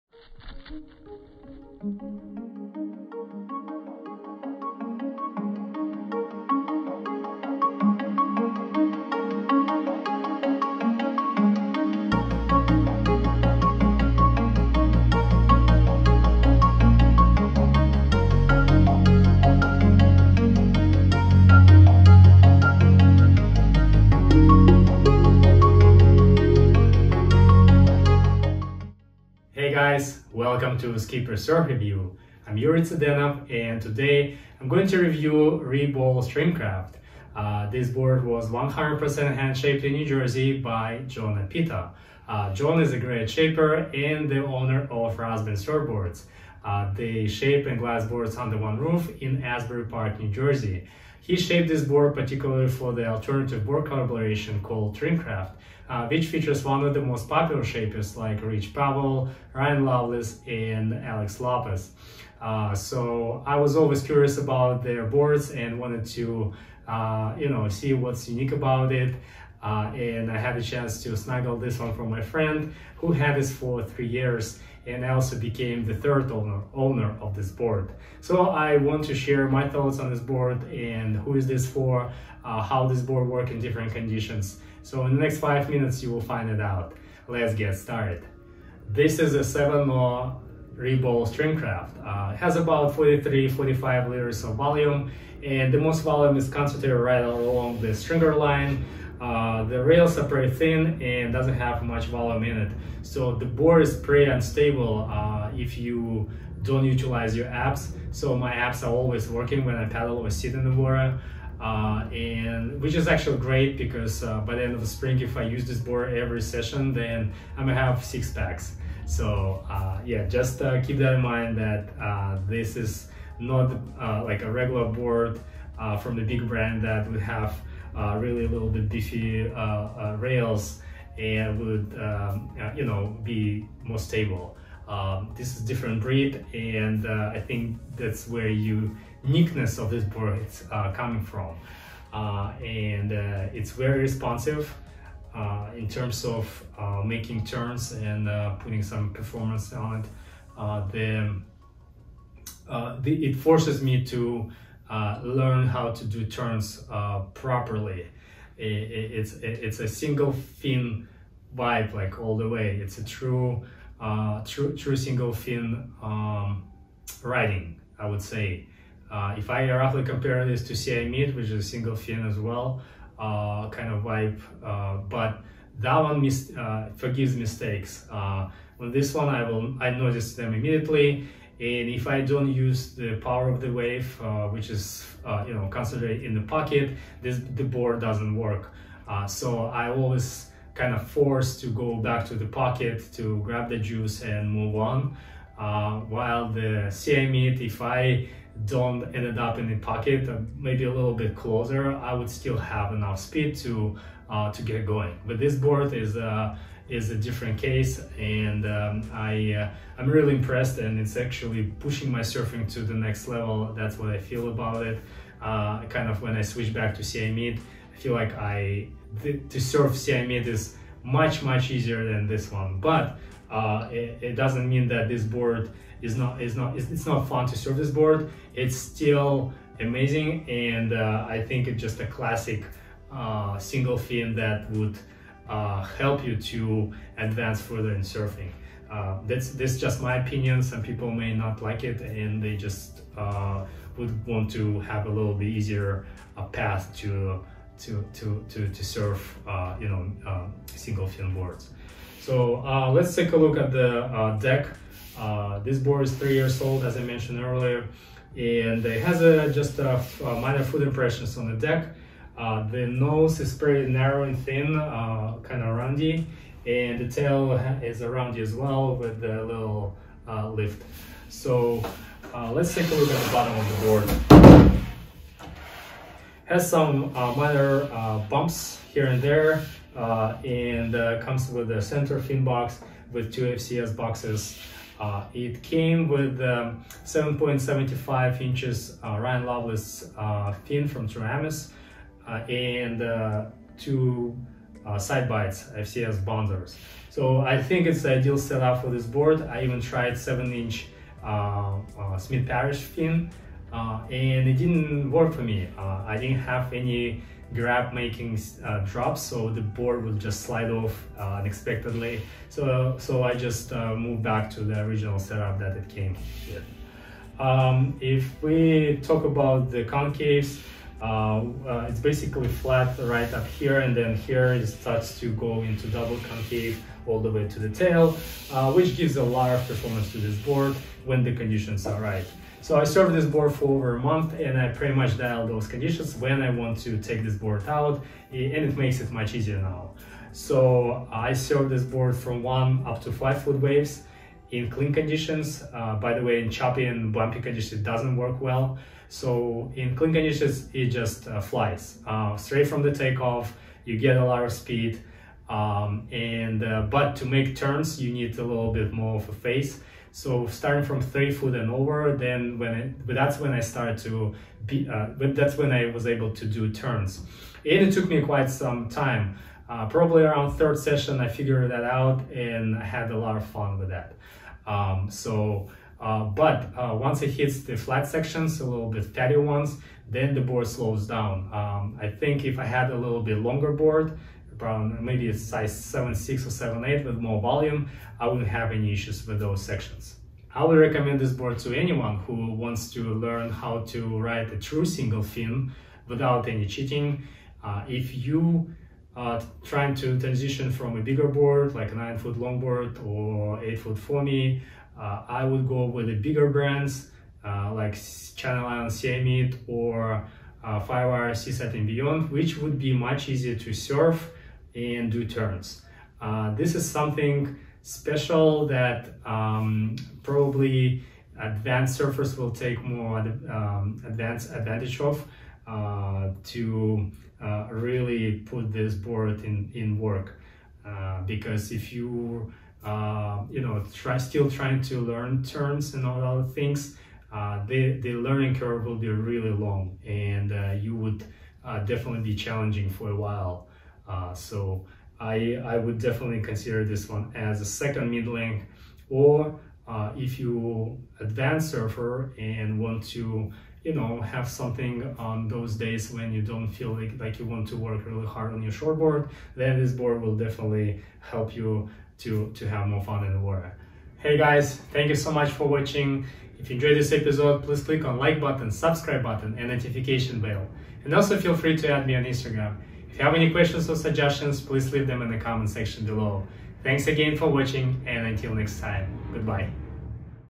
Trimcraft re.Bowls Surfboard Review
In this episode, I review Trimcraft re.Bowls. It’s a versatile high performance egg that delivers an organic single fin glide with a steep learning curve behind.

The board was hand shaped by John Oppito. John is a great shaper and the owner of Rozbern surfboards. Their boards get shaped and glassed under one roof in Asbury Park, NJ.

John shaped this board for the alternative-board collaboration Trimcraft which features designs from shaping Rich Pavel, Alex Lopez, and Ryan Lovelace.

I was always curious to ride one of those boards and I eventually had a chance to snuggle one from my friend.

I’ve been riding this board for over a month and all I can say is that the board will perform pretty well when the conditions are right. I pretty much dialed those conditions and now it makes it so much easier to make a decision on when I can take this board out.

I was having the most fun on it when the waves were from 1 up to 5 ft with clean conditions. It didn’t work well for me on choppy mushy days.

I didn’t have a chance to take it out on bigger days as those days happen once in 2 months and it’s hard to catch.
The ideal conditions start kicking in from 3 ft and over. That's when I was actually able to turn it which took me a few sessions to figure that out. After that, it was great. I was able to do basic maneuvers, cutbacks, floaters and cruising down the line with a lot of speed.

However, I’d say, It’s not as user friendly as CI Mid or Seaside and Beyond. It’s a different breed that wants to take time to build relationships and it doesn’t forgive your mistakes.

Board dimensions: re.Bowls 7’0” x 20 ¾ x 2 ¼
Volume: ~43-45L

My Height: 5’9” or 175.3 cm
My Weight: 155 lbs. or 70.3 kg

Filmed on DJI Mavic Air 2 / iPhone 12 Pro / Soloshot3 Optic 25 & 65